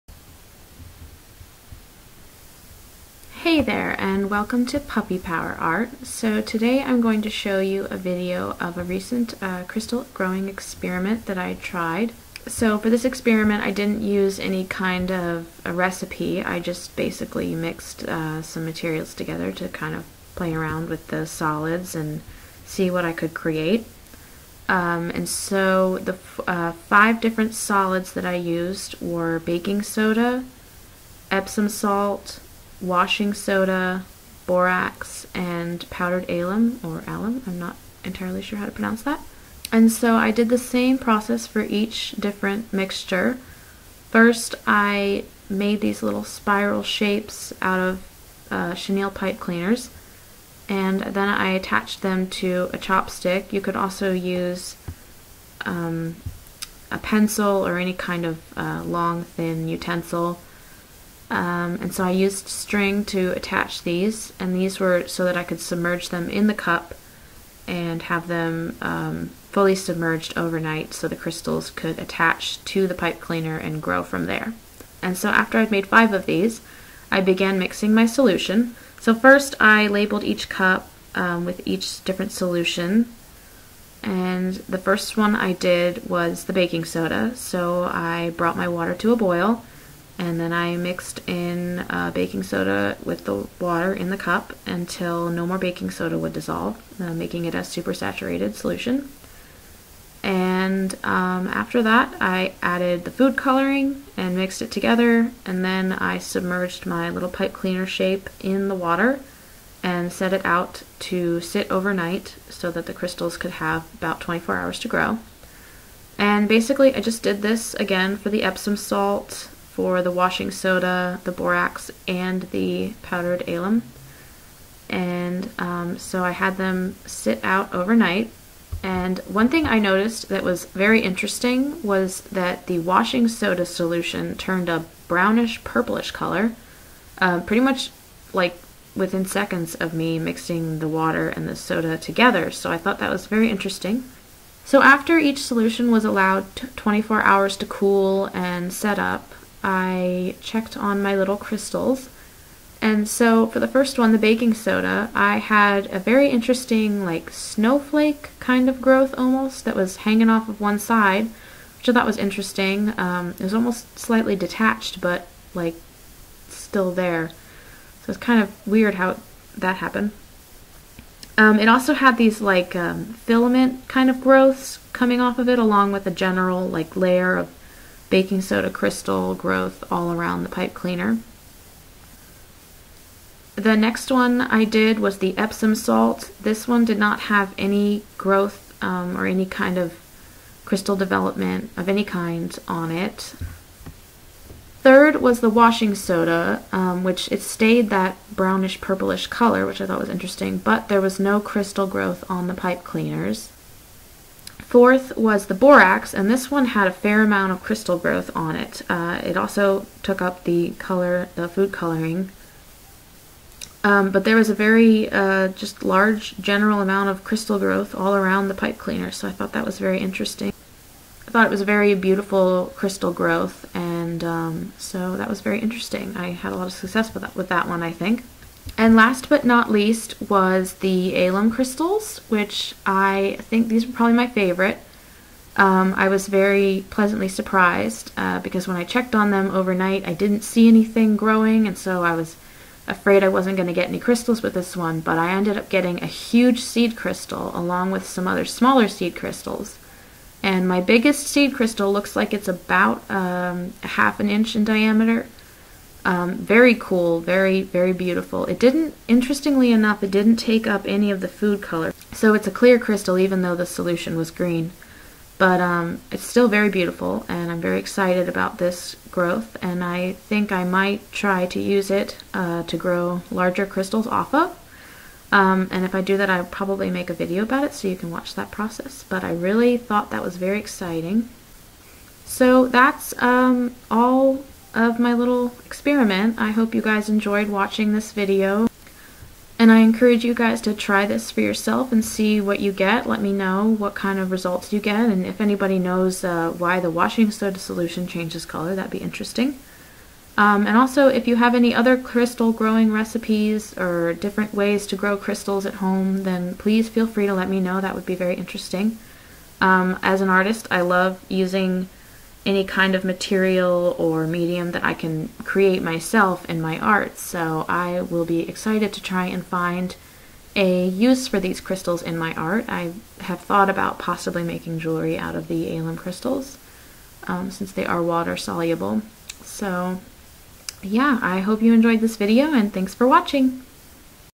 Crystal Growing Experiment
Here is a video documenting an experiment I did in growing crystal using five different solids (Baking Soda, Epsom Salts, Washing Soda, Borax, and Alum powder.